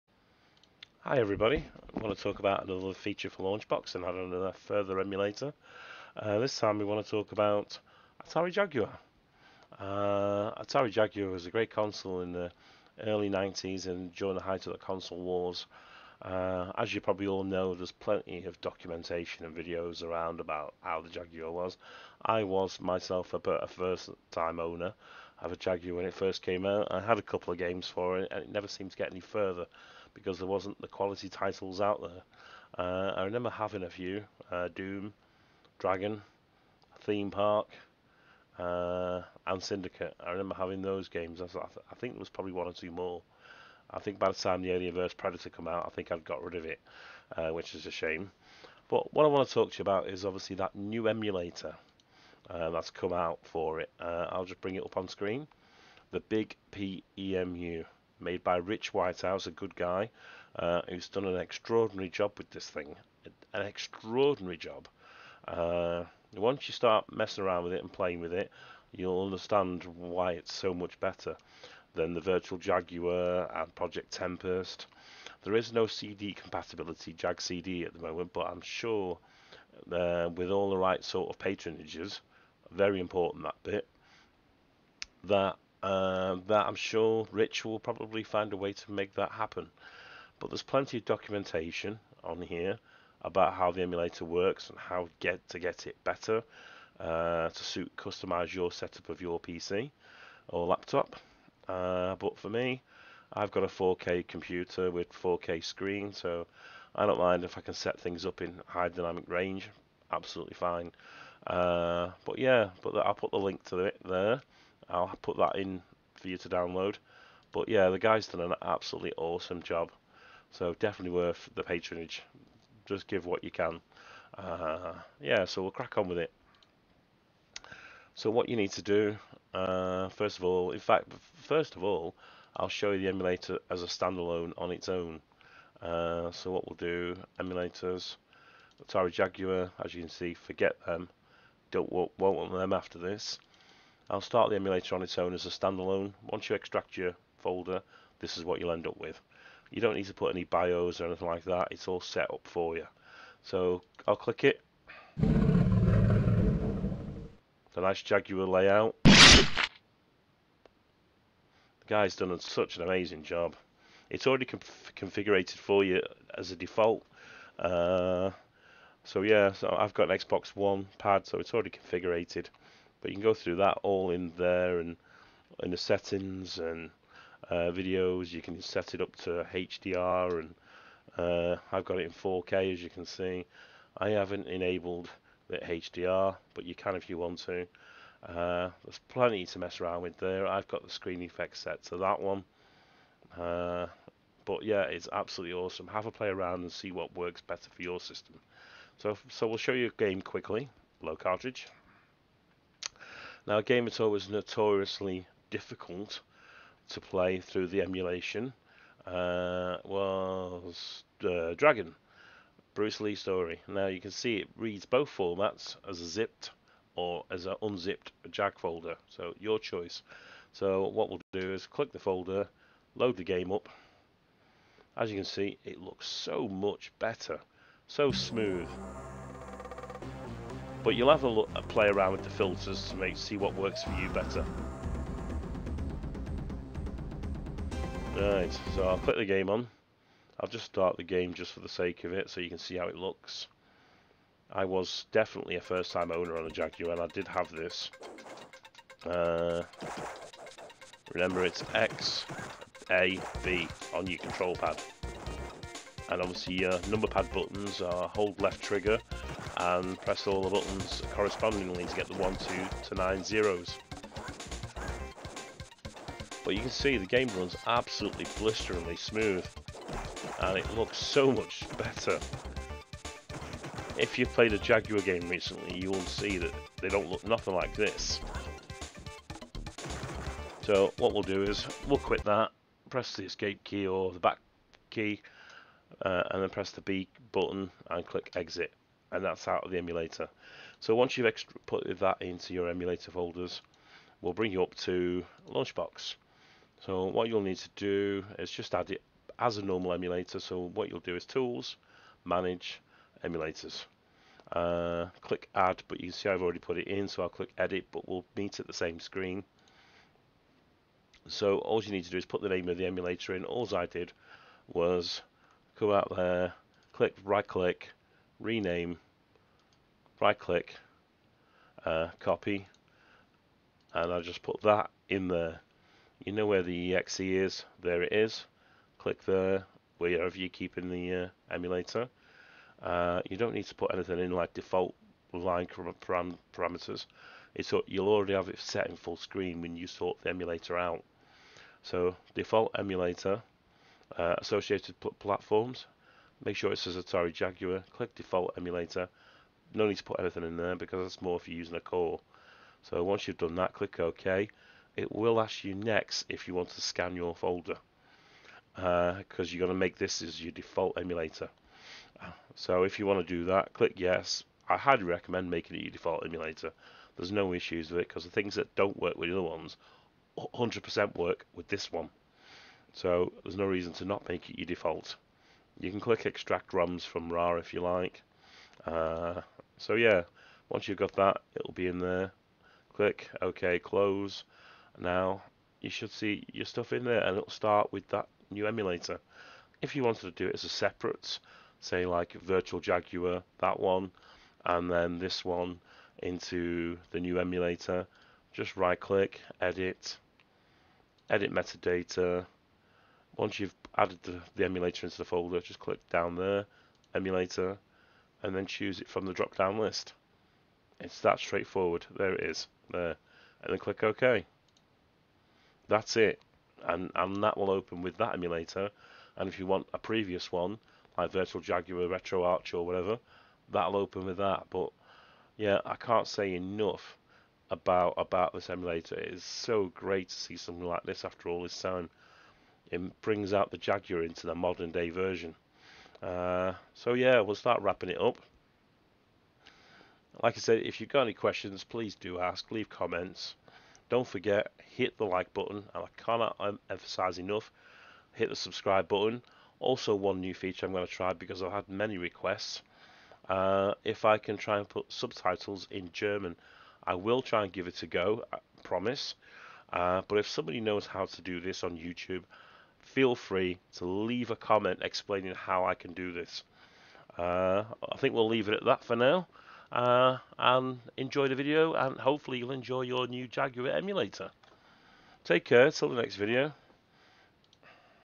Launchbox New 4K & HDR Atari Jaguar Emulator BigPEmu and setting it to Launchbox
Launchbox New 4K & HDR  Atari Jaguar Emulator BigPEmu and setting it to Launchbox any Questions leave a comment hit like and subs button